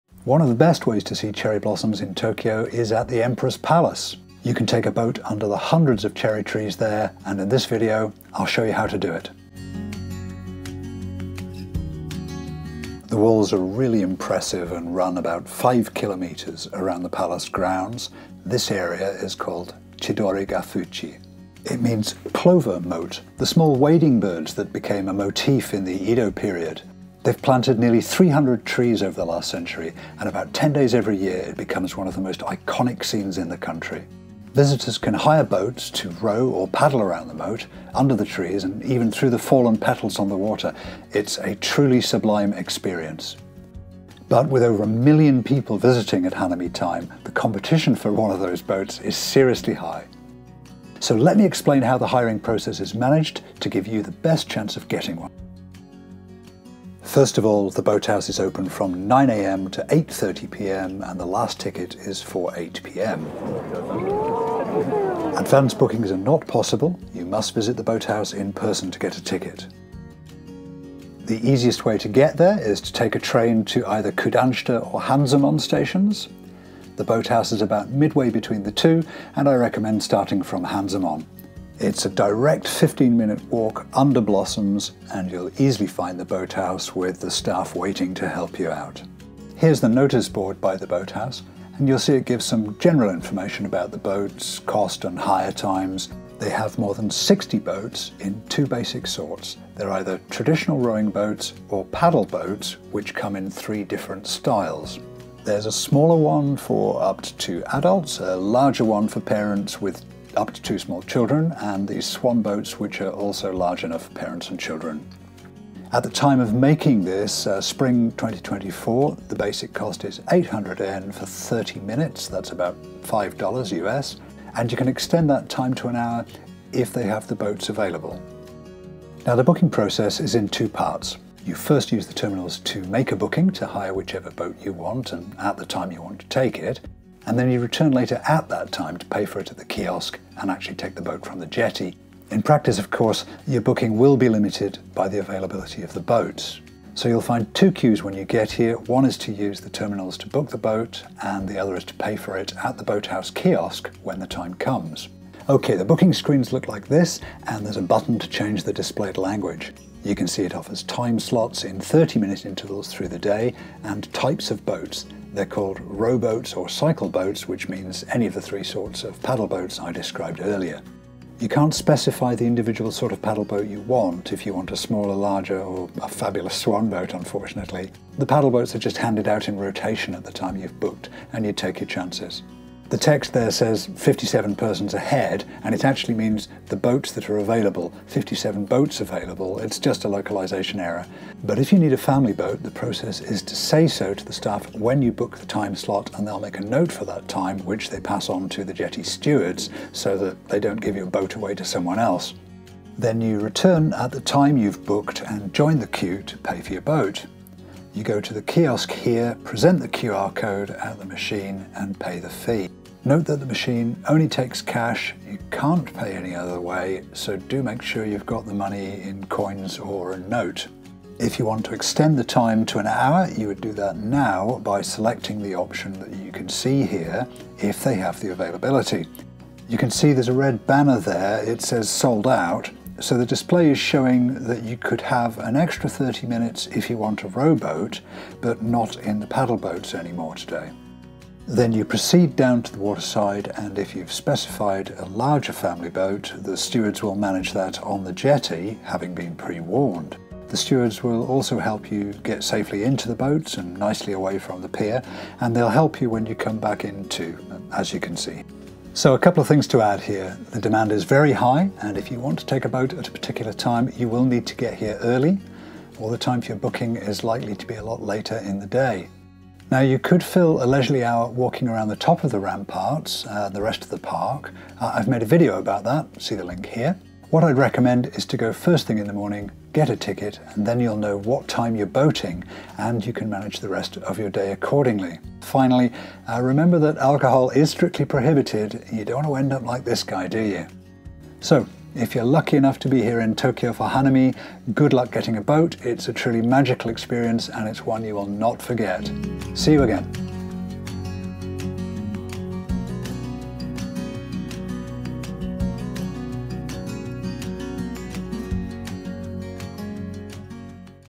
How to Rent Boats for Hanami in Tokyo
One of the most iconic views in Tokyo is of pleasure boats under cherry blossoms in sakura season, and this video shows how to book such a boat.

There's a map in the video showing the best route, from Hanzomon Station on the Hanzomon Line

The boat house is an extraordinary 15 minute walk through and under hundreds of some of the best cherry blossoms in the city, and the route has good access and is managable for elderly, disabled and wheelchair users.

Chapters
00:00 Titles
00:14 About the moat
00:58 Opening times
01:11 Getting there; route map
01:32 General info
01:39 Types of boats available
01:57 Cost
02:08 Booking process
03:24 Payment
04:02 Taking a boat
04:25 Notes and advice
04:51 Summary